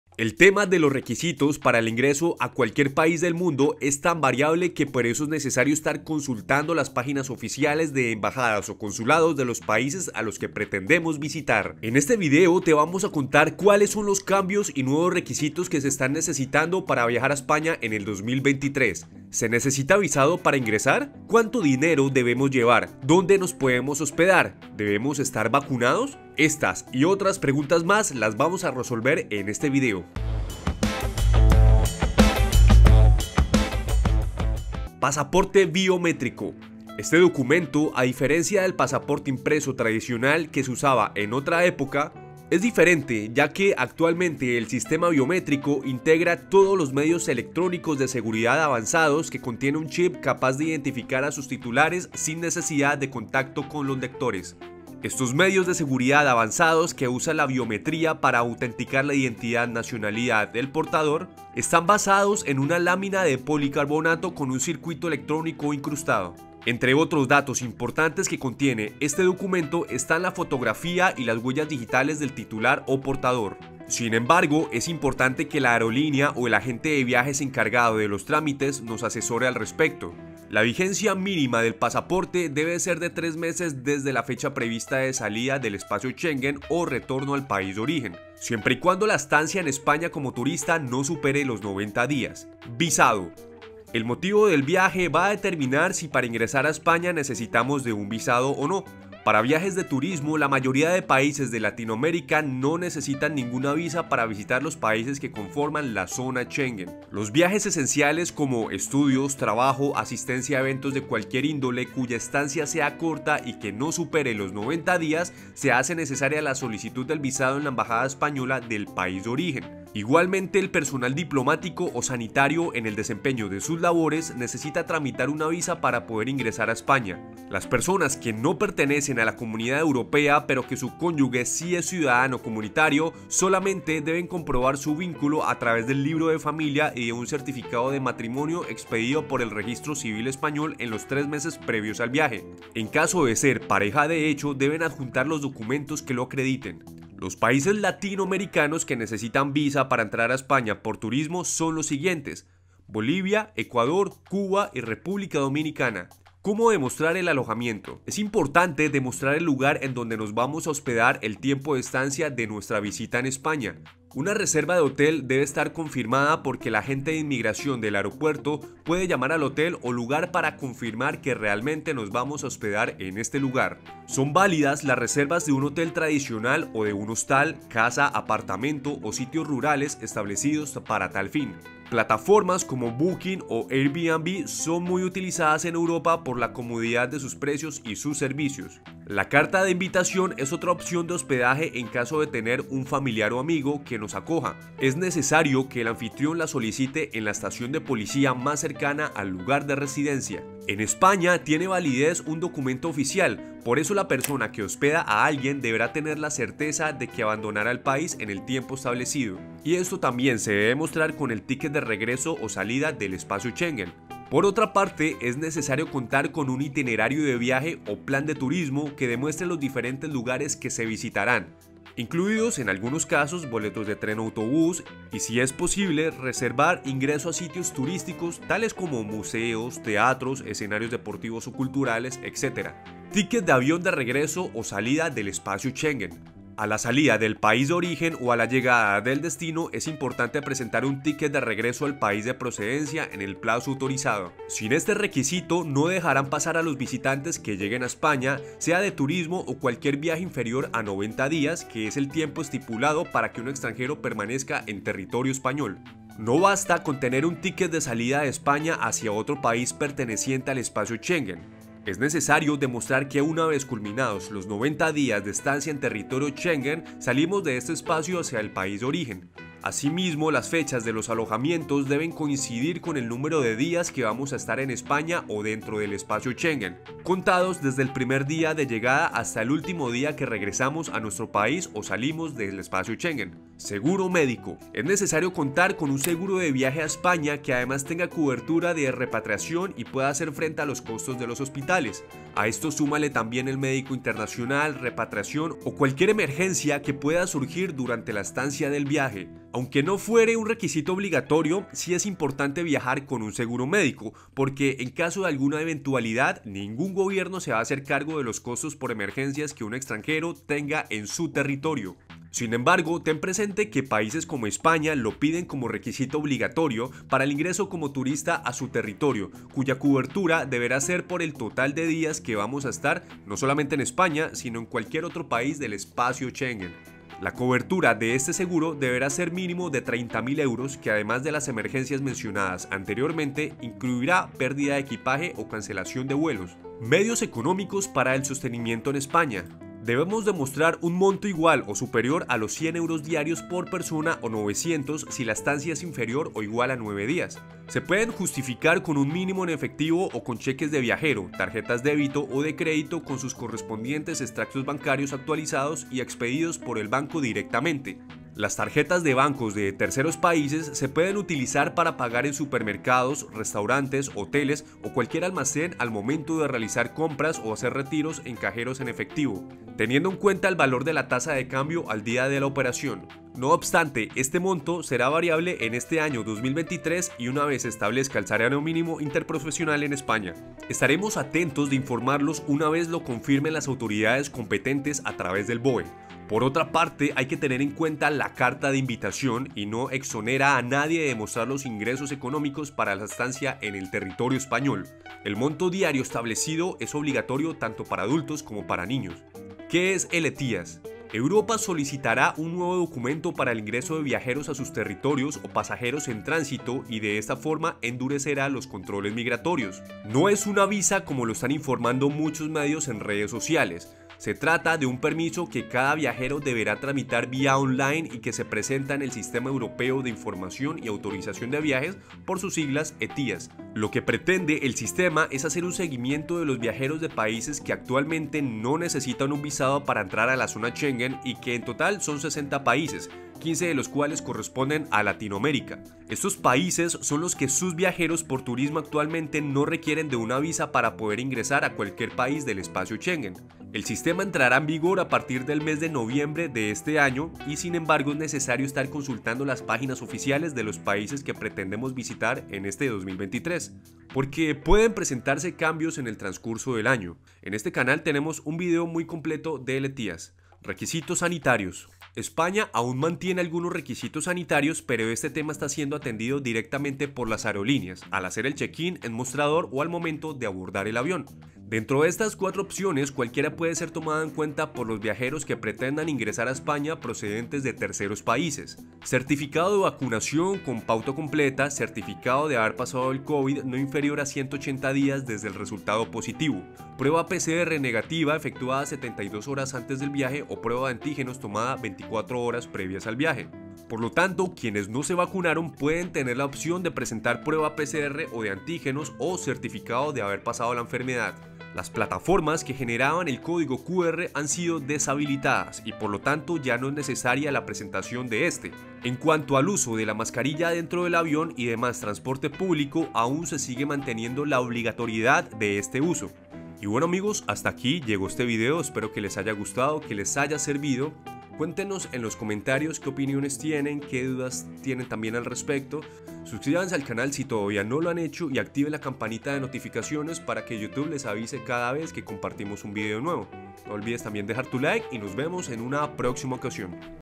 🚨 Qué NUEVOS requisitos se necesitan para VIAJAR a ESPAÑA 2024
En este video te vamos a contar cuáles son los cambios y nuevos requisitos que se están necesitando para viajar a España en este 2023.

¿Se necesita visado para ingresar? ¿Cuánto dinero debemos llevar? ¿Dónde nos podemos hospedar? ¿Debemos estar vacunados?. estas y otras preguntas más las vamos a resolver en este video.

Allí también encontrarás nuestro libro CÓMO VIAJAR A ESPAÑA SIENDO LATINOAMERICANO / GUÍA PRÁCTICA

Mira este video hasta el final ya que toda la información que aquí te brindamos pasa por un riguroso proceso de investigación, con el fin de brindarte el mejor contenido para planear bien tu viaje.